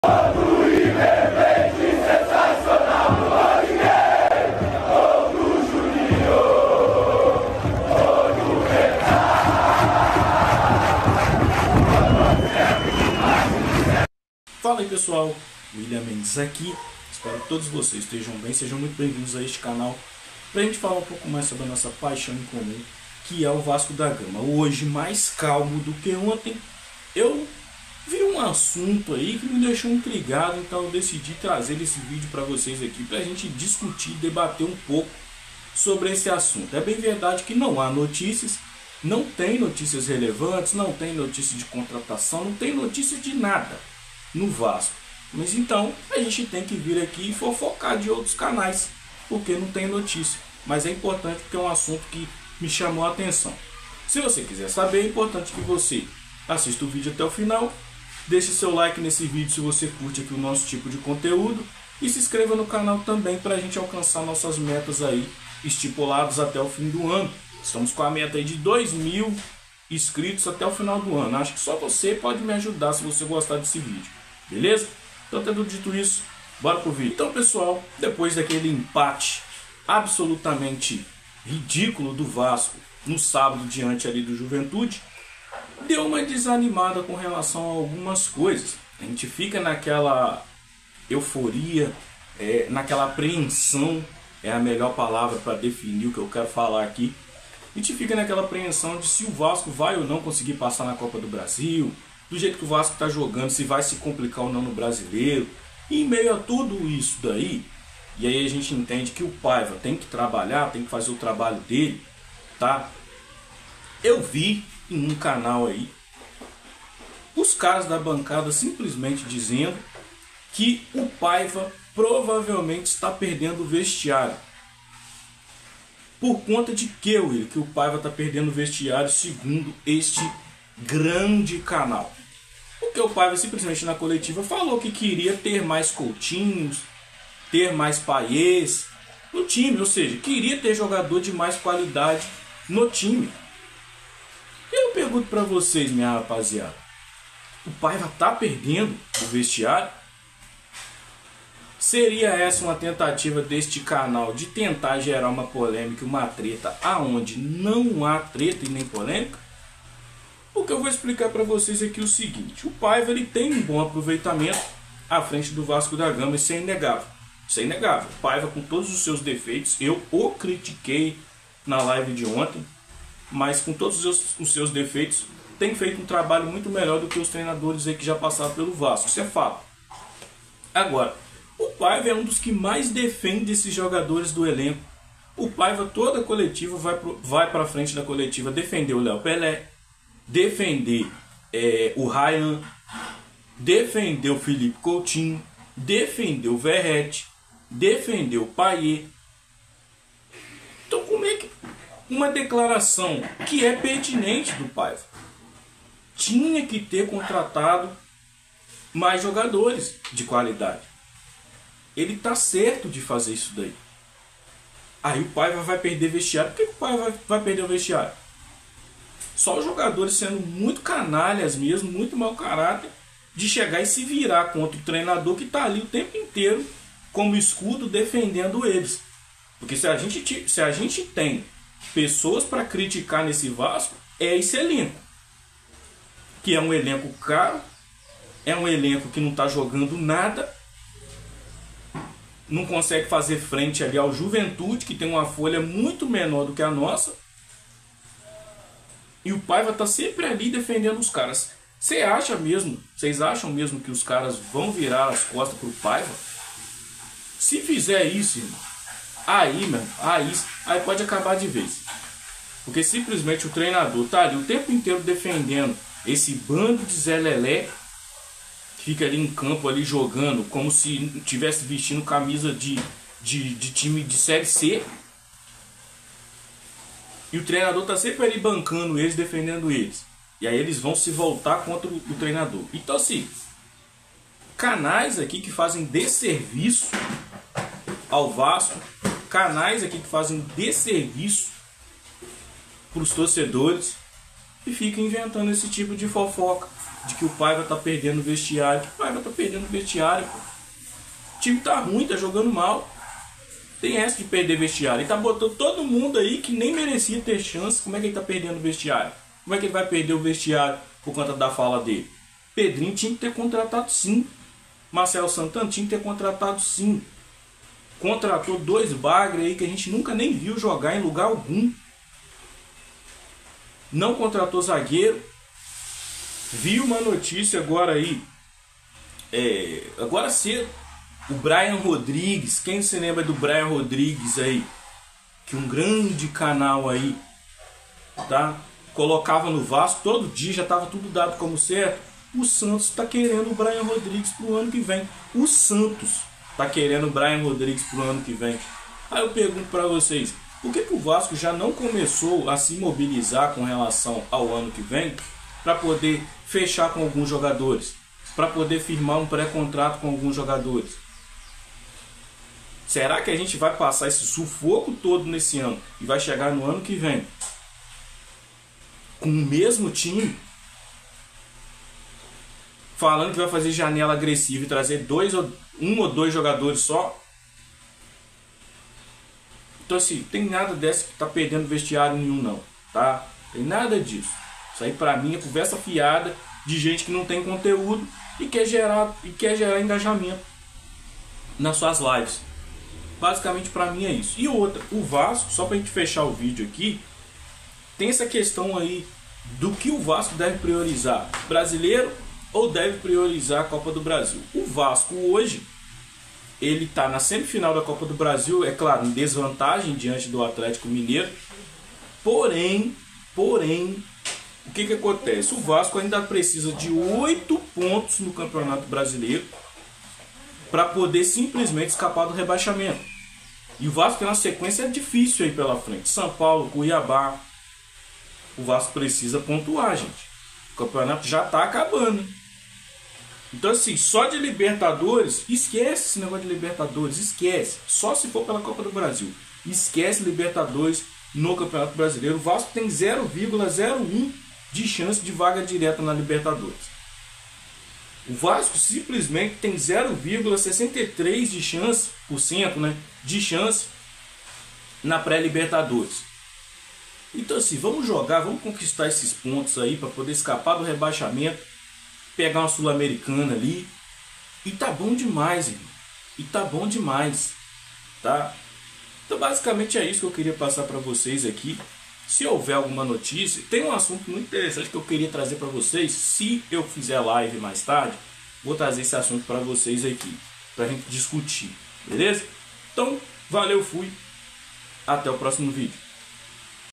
URGENTE! PAIVA PERDEU O VESTIÁRIO? DISPAROU CONTRA A DIRETORIA!
Rafael Paiva vive momentos de incerteza no Vasco e já estão dizendo por aí que ele perdeu o vestiário.